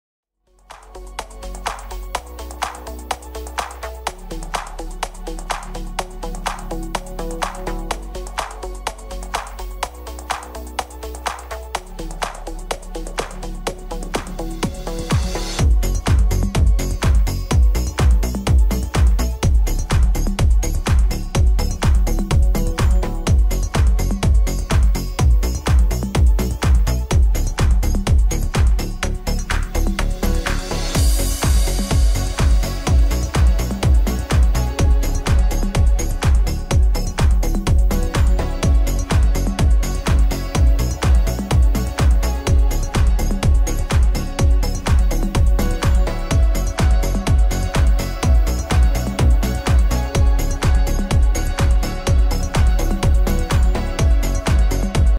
Bloquer les pub sur android oppo | Comment arrêter les publicités sur mon téléphone oppo
Comment supprimer les publicités sur mon portable oppo. Сomment bloquer les annonces sur mon telephone oppo